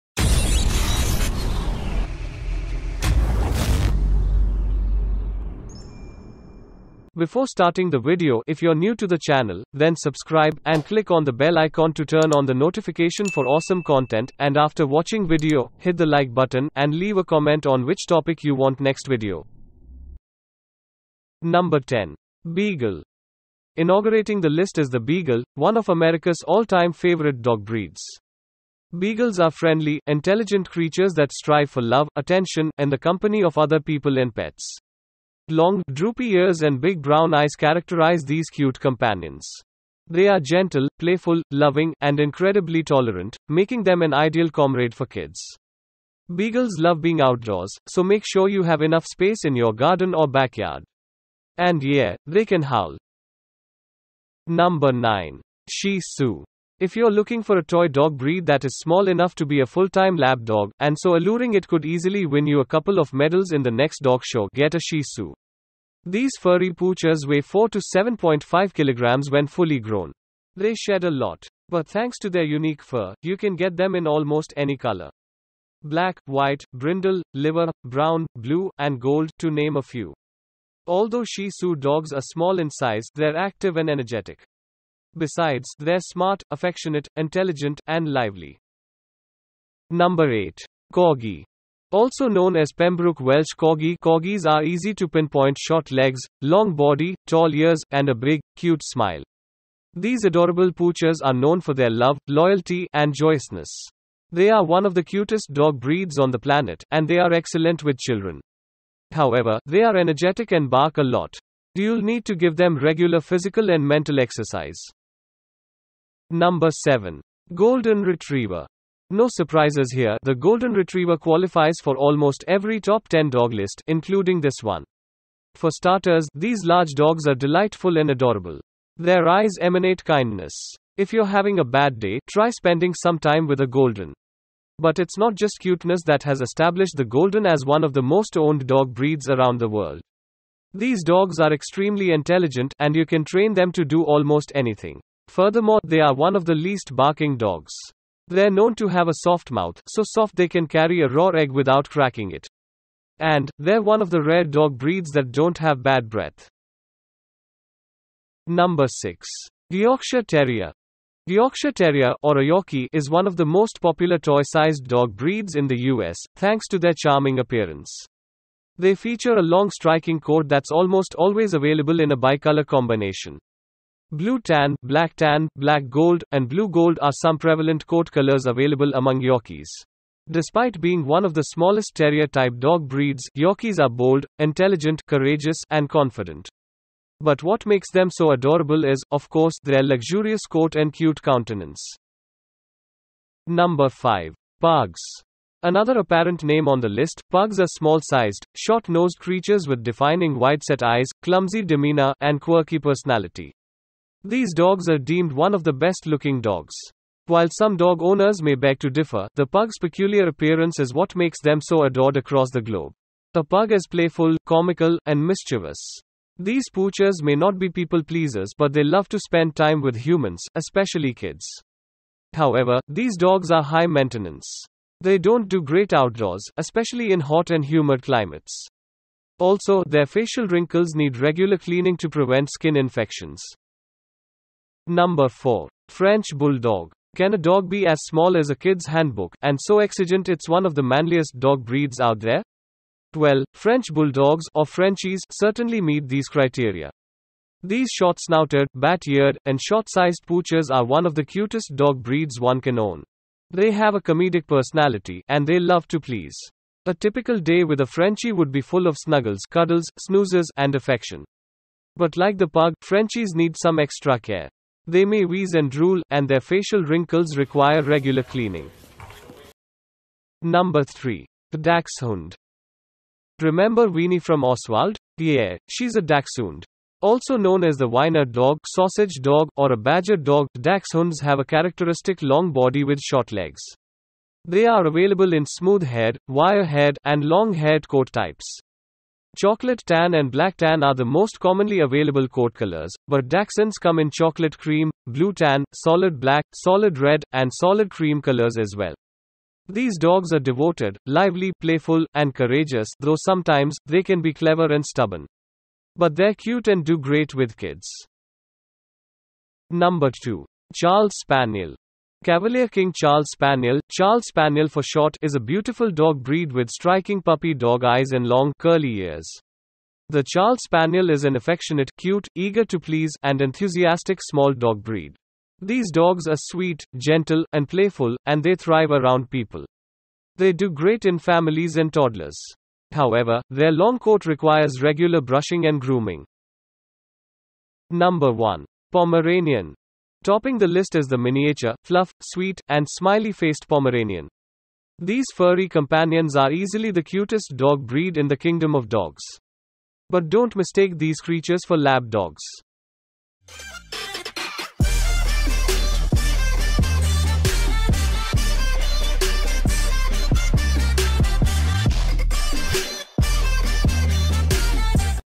Top 10 Cutest Dog Breeds to Keep as Pets
A man's best friend: Dogs. Those little hairy pups are the ones that always make our lives happier with their occurrences.
Every pet parent thinks that their dog is the cutest pooch in the world. Here’s the thing…they’re right! Big or small, fuzzy or fluffy, yappy or chubby, we know your four-legged bestie is probably one of the cutest dog breeds you’ve ever laid eyes on. That’s just how dogs work. They are born to be adorable!

Come on now, you know you want an excuse to look at our Top 10 of outrageously cute pups! We dare you not to say “AWWWWWW!”

In this video, we are counting down our top picks on Top 10 MOST Cutest Dog Breeds!